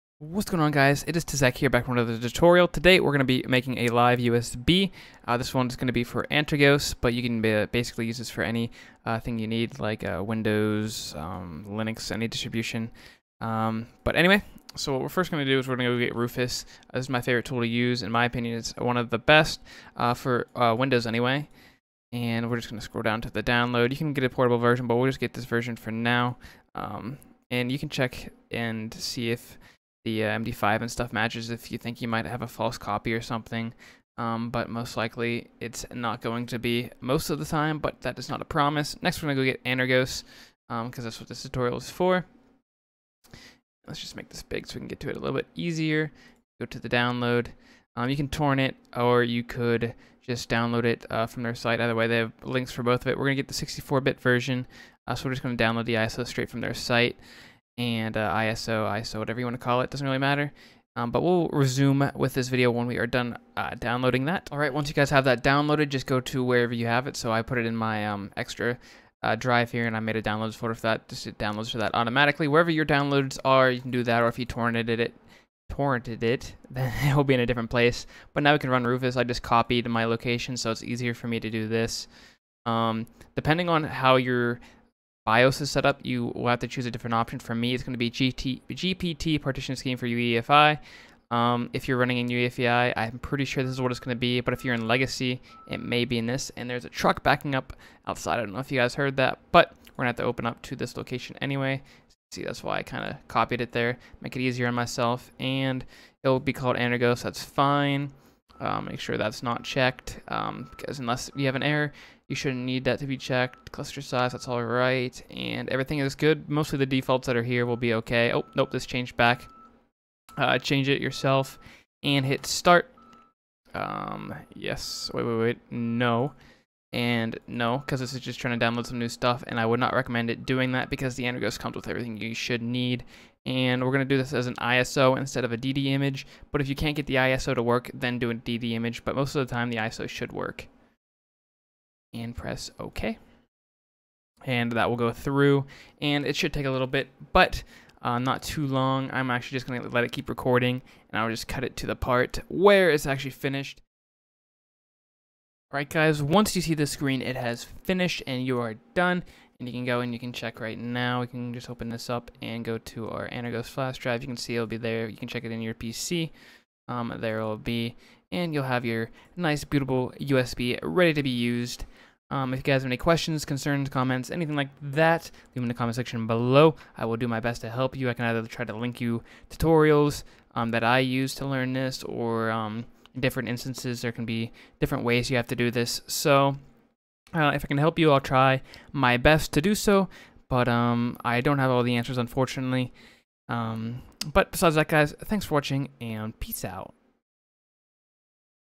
How to Make a Live Antergos USB on Windows 10 - Rufus (UEFI)
Today I will teach you how to make a Live USB of Antergos with Rufus. This tutorial is intended for people using UEFI in their BIOS settings and are running Windows 10.

Antergos is a is a Linux distribution based upon Arch Linux.

PC Setup:
CPU Cooler
Power Supply